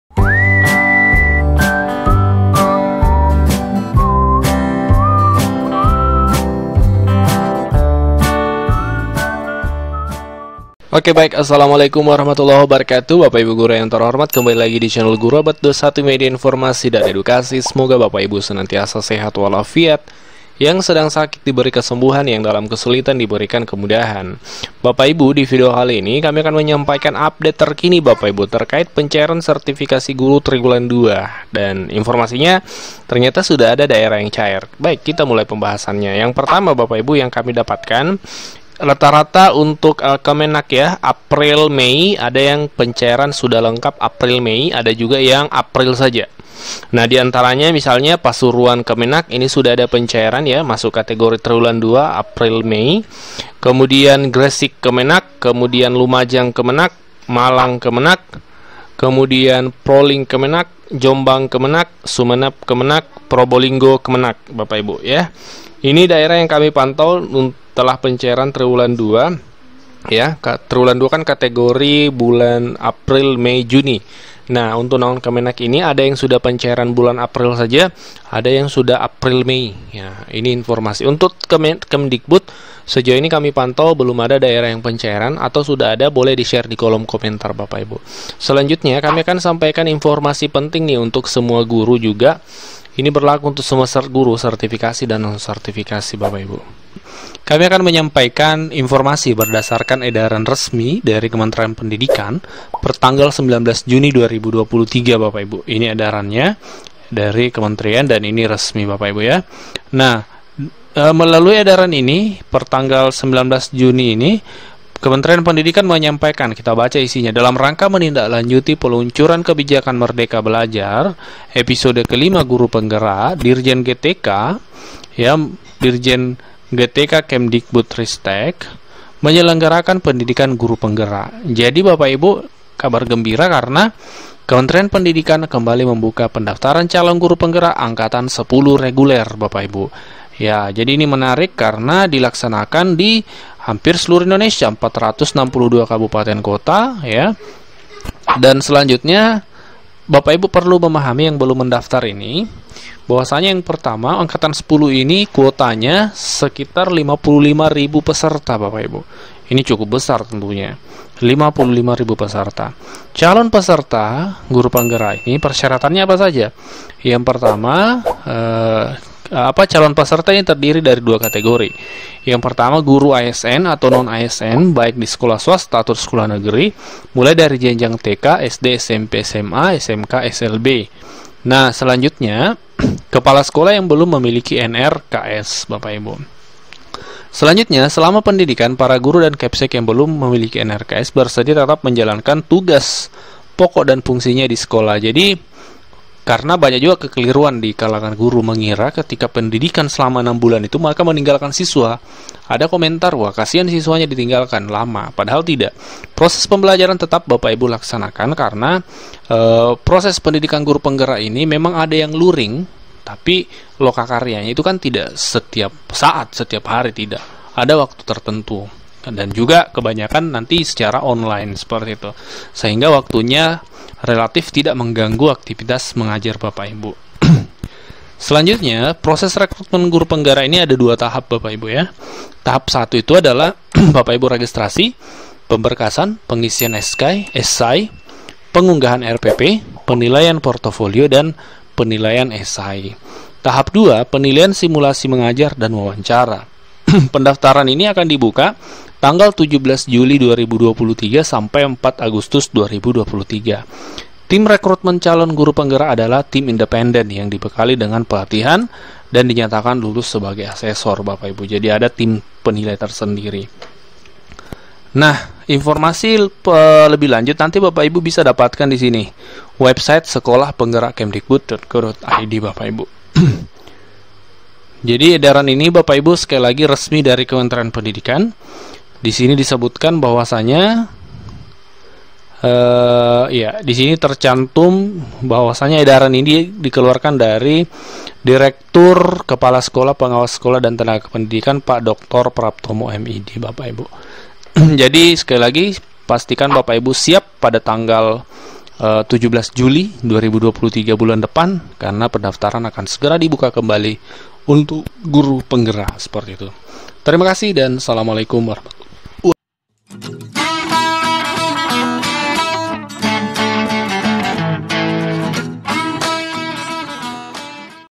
Horee! Kabar gembira hari ini pencairan tunjangan sertifikasi guru triwulan 2 tahun 2023
Ini adalah kabar gembira dari pemerintah perihal pencairan tunjangan sertifikasi guru triwulan 2 tahun 2023